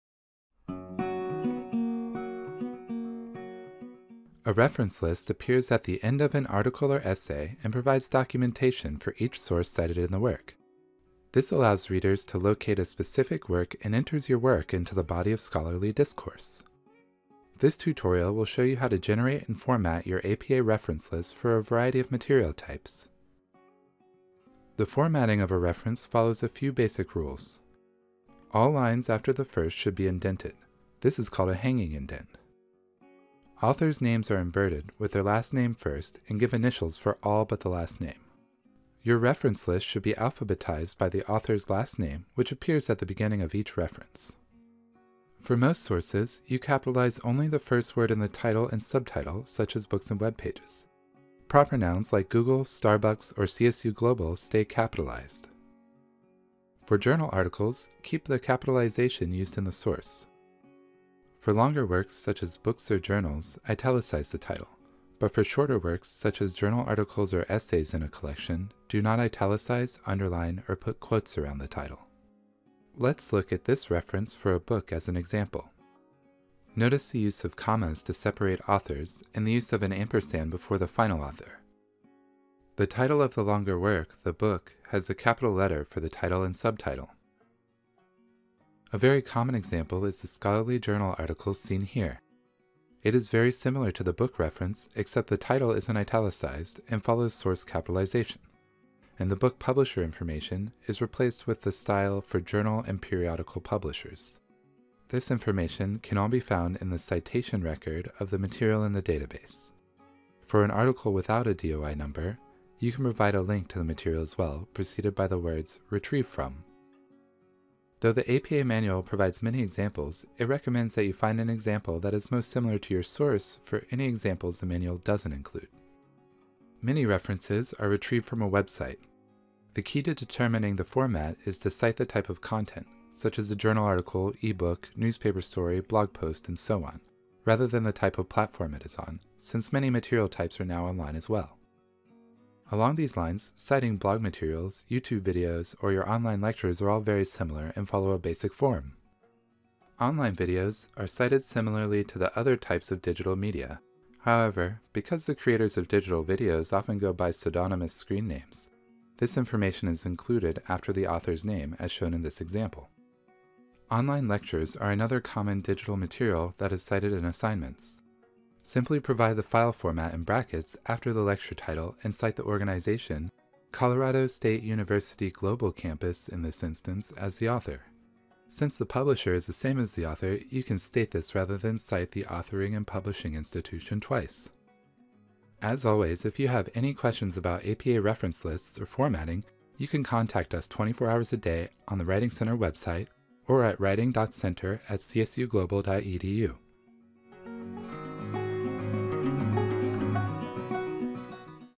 APA Reference Lists - CSU Global Writing Center
A reference list appears at the end of an article or essay and provides documentation for each source cited in the work. This allows readers to locate a specific work and enters your work into the body of scholarly discourse.

This tutorial will show you how to generate and format your APA reference list for a variety of material types.